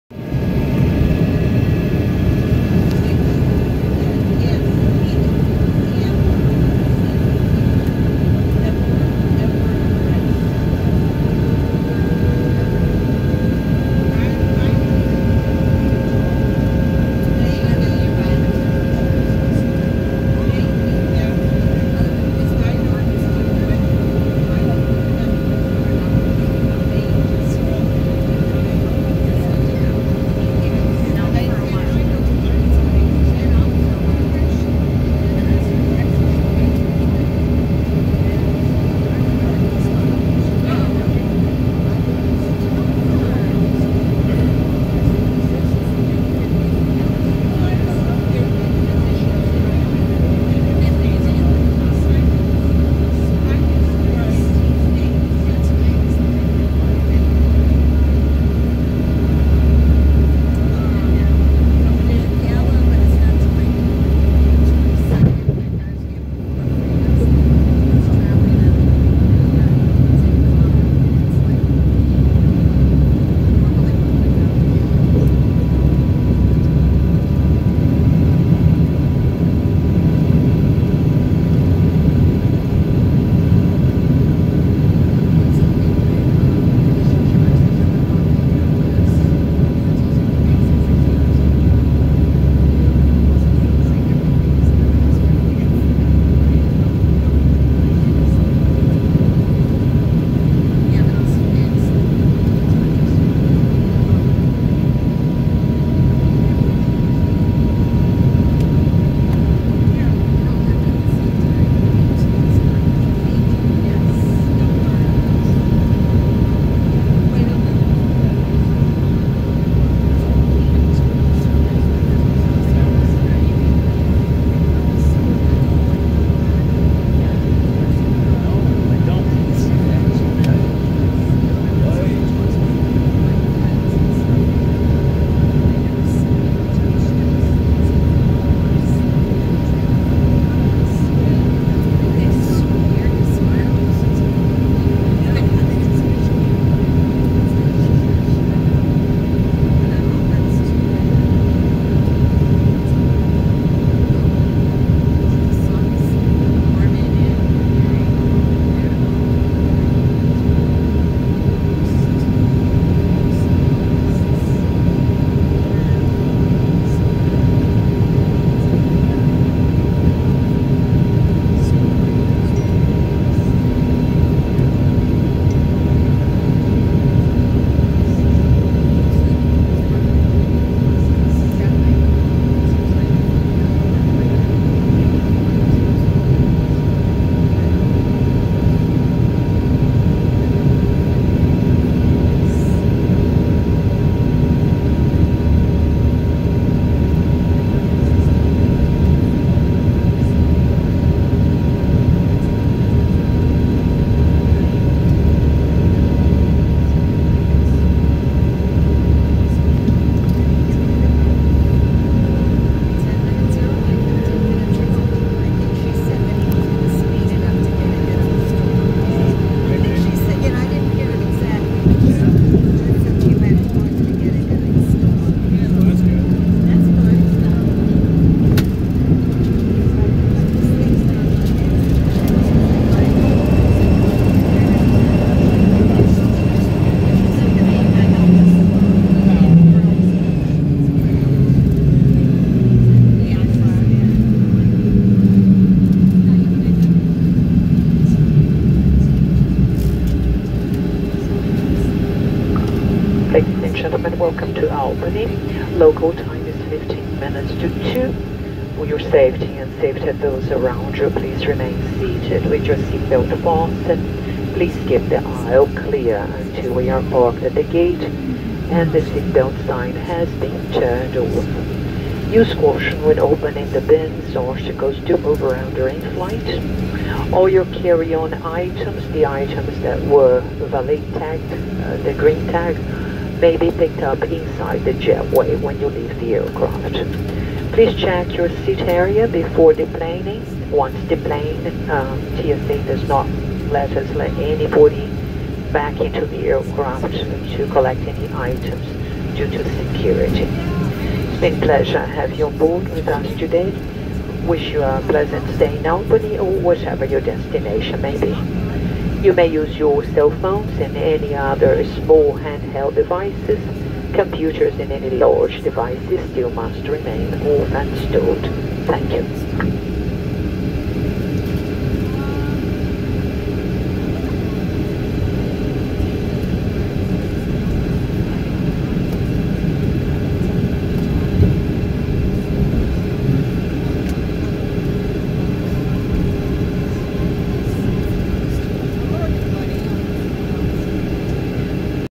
Landing United Airlines CRJ Albany Airport ALB
Landing of United Airlines CRJ 200 at Albany Airport ALB. Flight from Washington Dulles IAD. Flight operated by Air Wisconsin. Flight UA 3811.